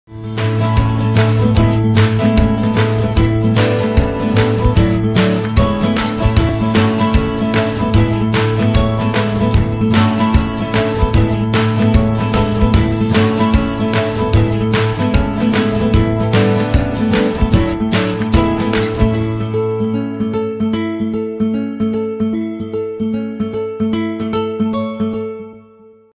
Coffee bar packaging video
Disposable wooden coffee bars, including a variety of specifications and different quality, to provide you with a variety of choices.